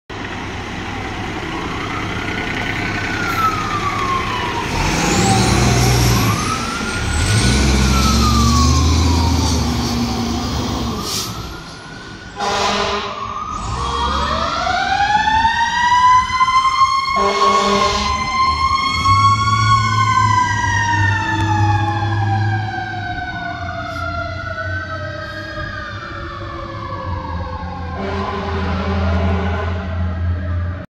Mount Gilead Fire Department Rescue 11 Responding
Mount Gilead Fire Department Rescue 11 Responding to a Motor Vehicle Accident